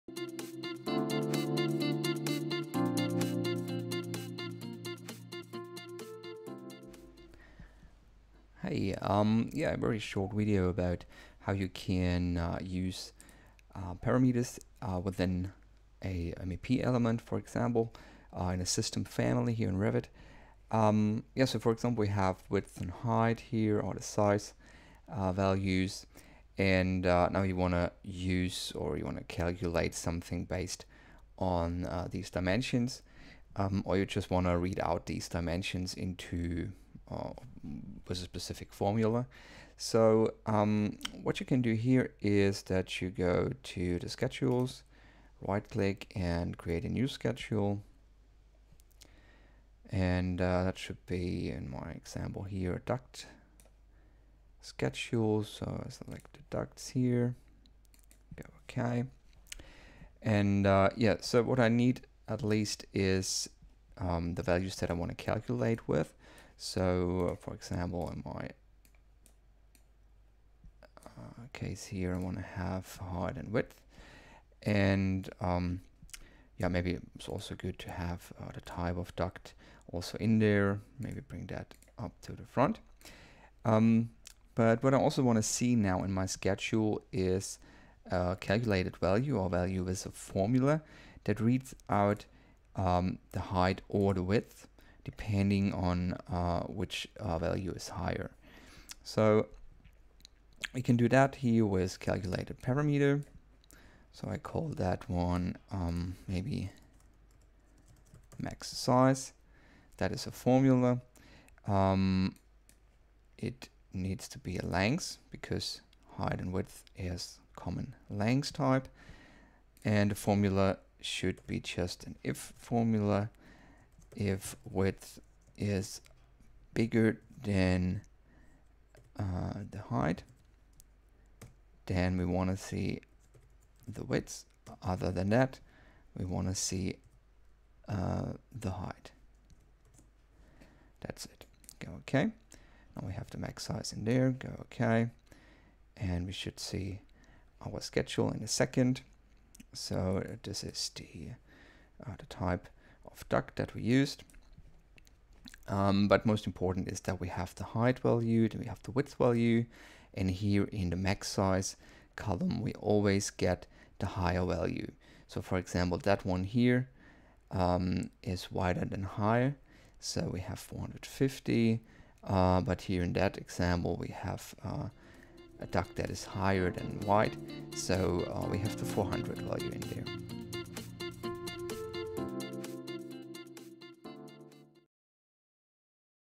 Autodesk Revit - Calculated Values in Schedules
With calculated values in Schedules you can use Parameters of e.g. Systemfamilies and use them for claculations or in a formula.